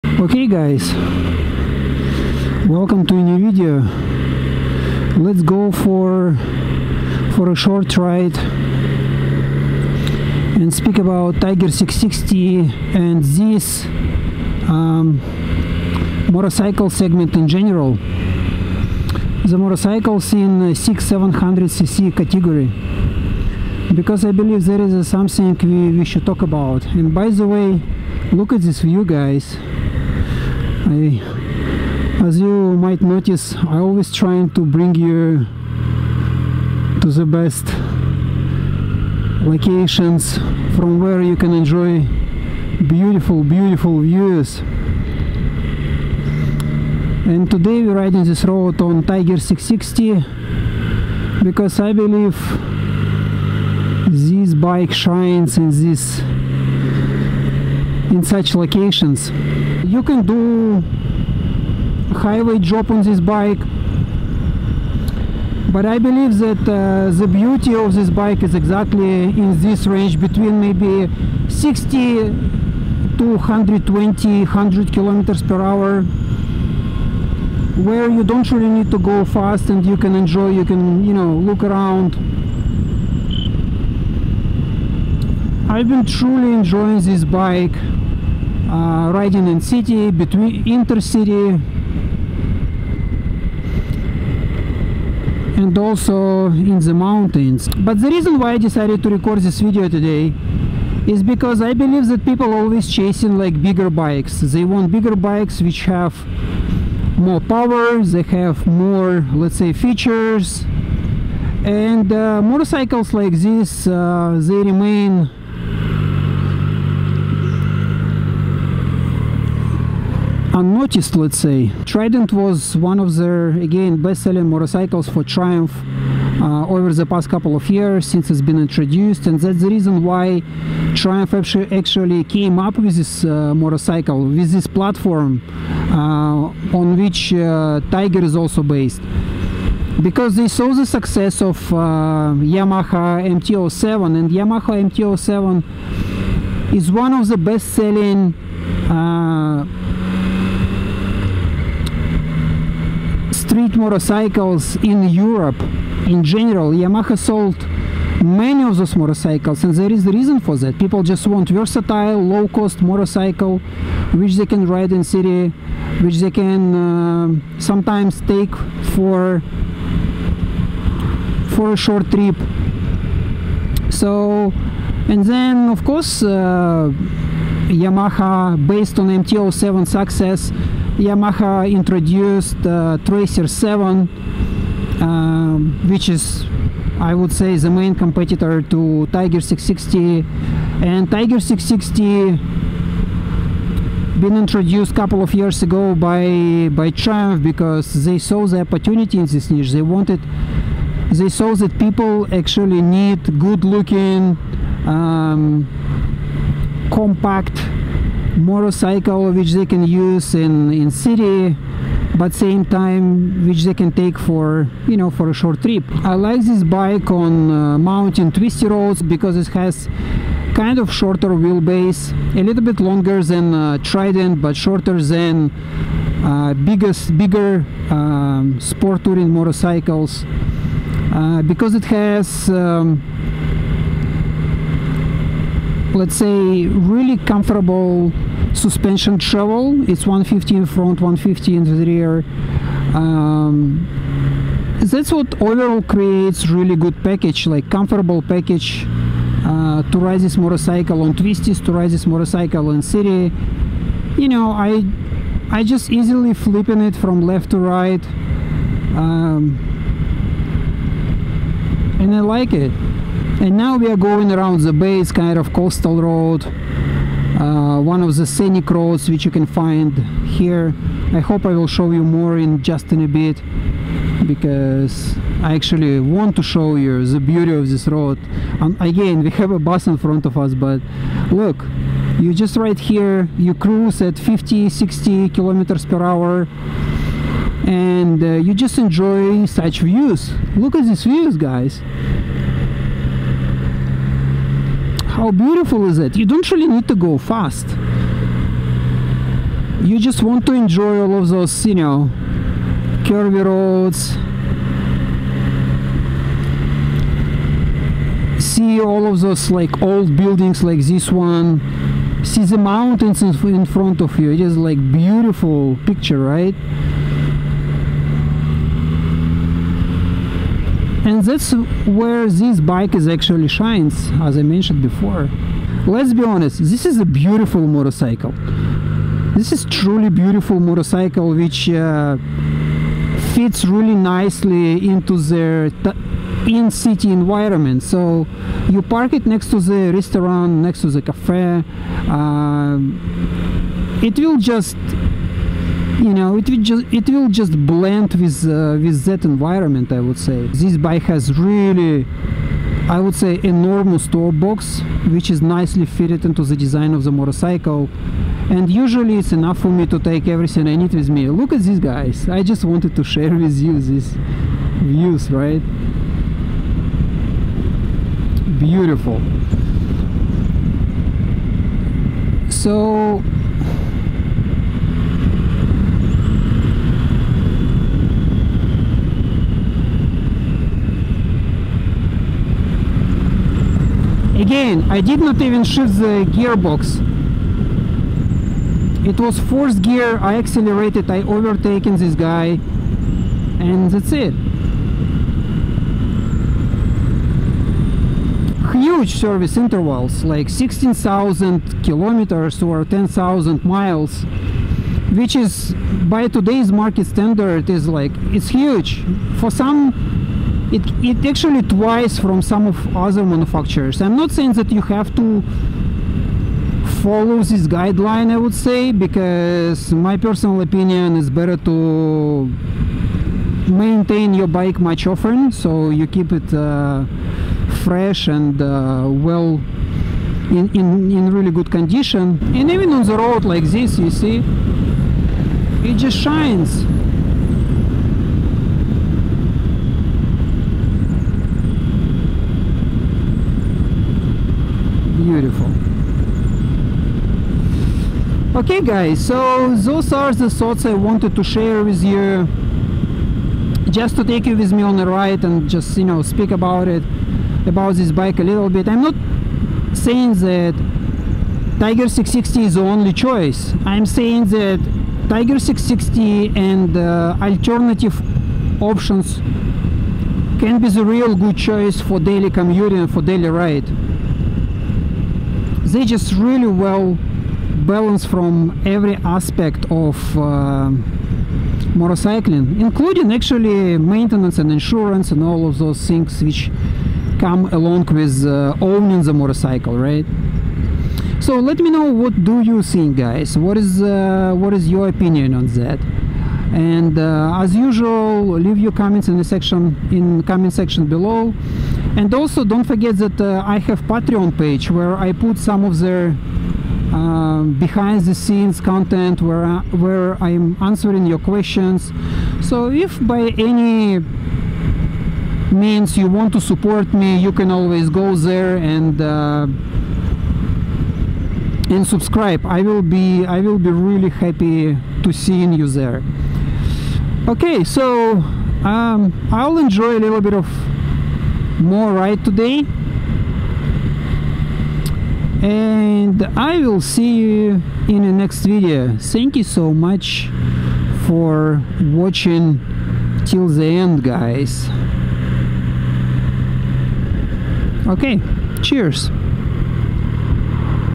Is Triumph Tiger 660 Sport the most overlooked bike?
Middleweight motorcycles like the Triumph Tiger Sport 660 are often overlooked in today's quest for bigger and more versatile bikes. However, these simple yet practical bikes can be the perfect solution for many riders. In this video, I'll explore the reasons why the Triumph Tiger Sport 660 is an excellent choice for riders seeking a versatile and reliable motorcycle.

You can buy footage from this video on my Shutterstock profile